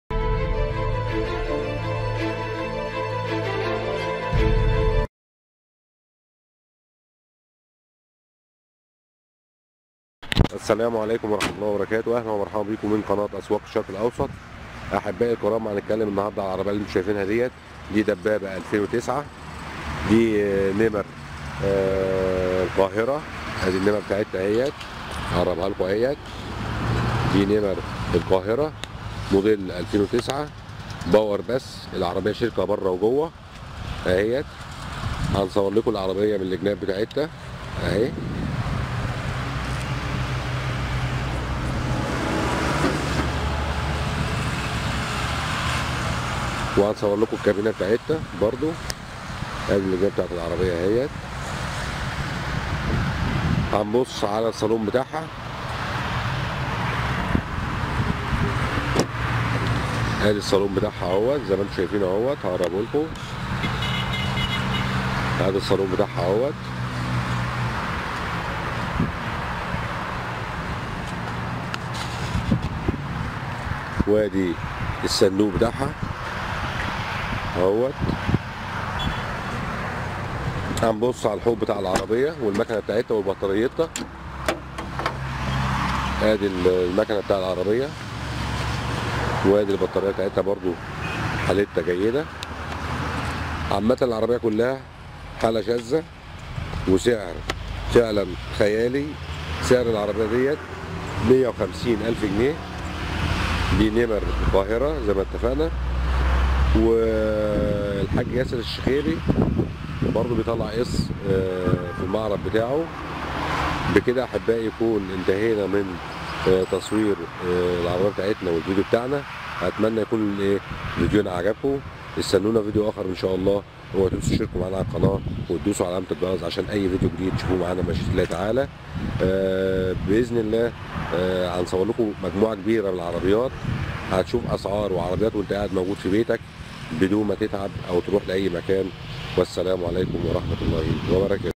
شيفرولية دبابة موديل 2009 للبيع في مصر السيارة بحالة زيرو والبيع كاش وقسط
سيارة شيفرولية بحالة فوق الممتازمع ان الموديل الخاص بهذة السيارة قديم بعض الشىء انما تجد ان حالة السيارة ممتازة من الخارج والداخل وممكن ان نقوم ببيع شيفرولية دبابة نصف نقل كاش وقسط والتسليم فوري